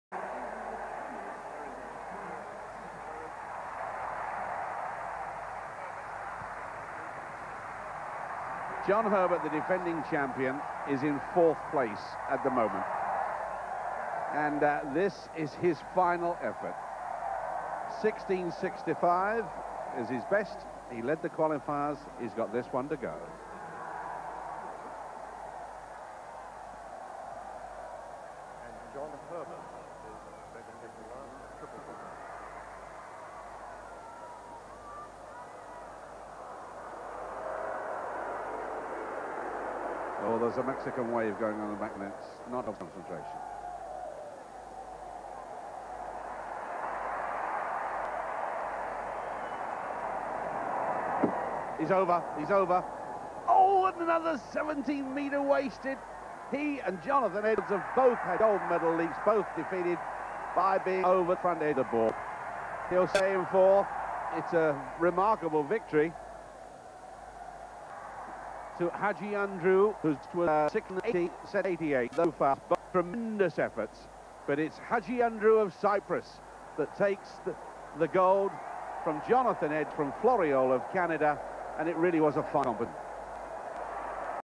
2667 Commonwealth Track & Field 1990 Triple Jump Men John Herbert
1990 Commonwealth Auckland Triple Jump Men John Herbert NJ
Final - 3 Feb 90
  1 Marios Hadjiandreou CYP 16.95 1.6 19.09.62
  2 Jonathan Edwards Eng 16.93 w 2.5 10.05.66
  3 Edrick Floreal CAN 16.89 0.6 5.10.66
  4 John Herbert Eng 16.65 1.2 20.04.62
  5 Festus Igbinoghene NGR 16.65 w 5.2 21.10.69 U23
  6 Andrew Murphy AUS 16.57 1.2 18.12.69 U23
  7 Vernon Samuels Eng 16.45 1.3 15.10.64
  8 Paul Nioze SEY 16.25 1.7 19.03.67
  9 Matt Sweeney AUS 15.99 w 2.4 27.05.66
  10 Mohammed Zaki Sadri MAS 15.78 w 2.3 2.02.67
  11 Brian Wellman BER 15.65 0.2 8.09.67
    George Wright CAN nm